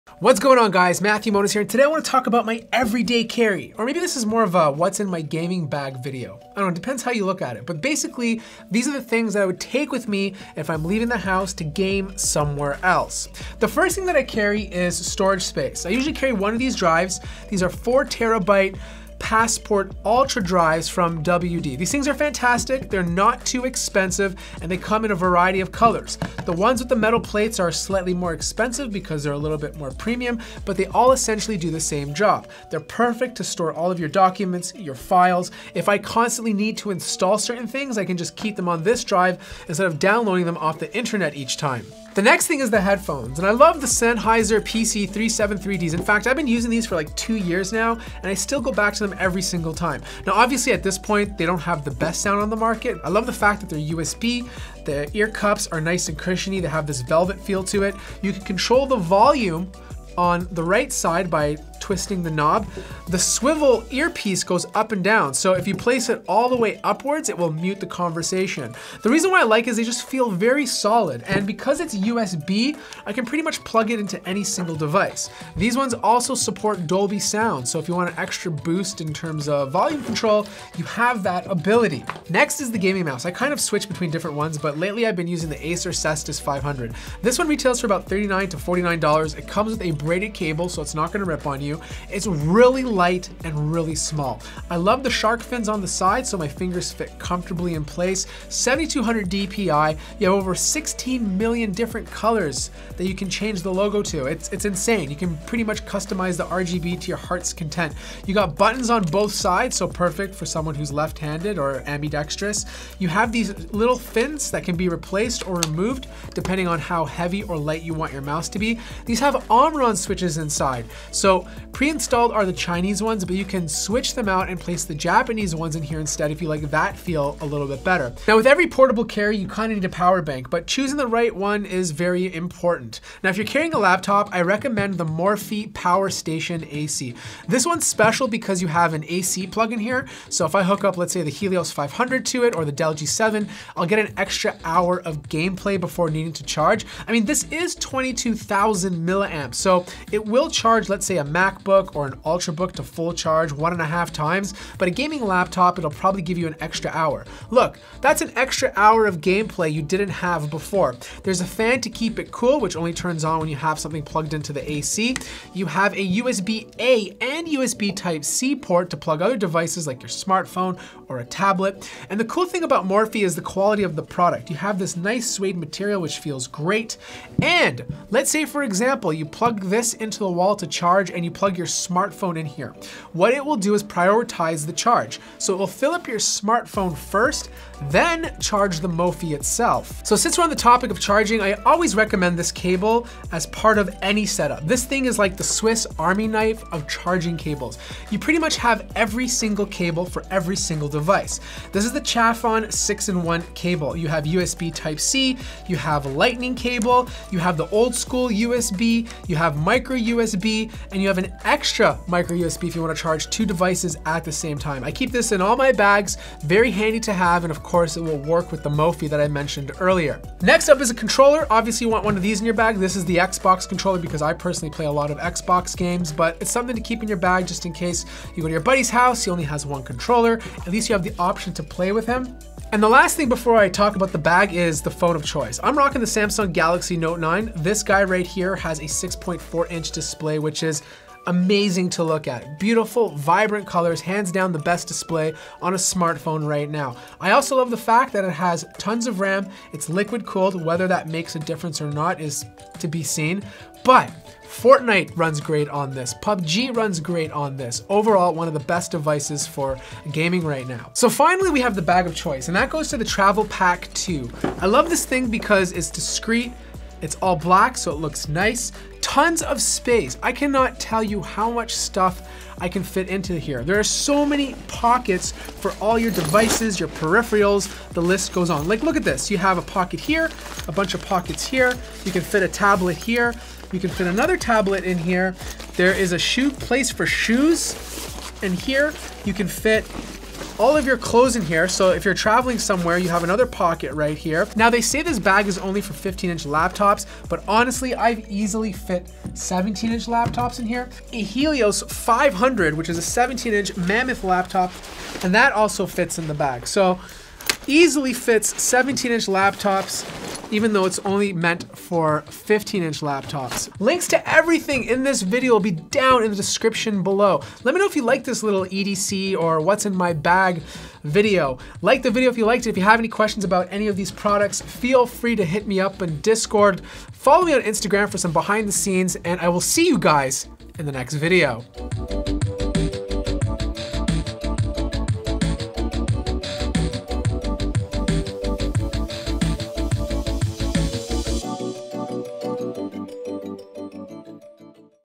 My Gaming EDC // Every Day Carry 2018! 💪
Here's what I carry when I'm Gaming or If I was gaming Everyday! I guess this is a EDC 2018 / What's in my bag combo video. Everything from Gaming Laptop to my smartphone daily driver!

WD My Passport Ultra HD
Chafon 6 in 1 Cable
Mophie Powerstation AC
Xbox Wireless Controller
Acer Cestus 500 Mouse
Sennheiser PC373D Headset

Livestream on YouTube every Friday at 4PM EST. Twitch livestream every Sunday at 9PM EST.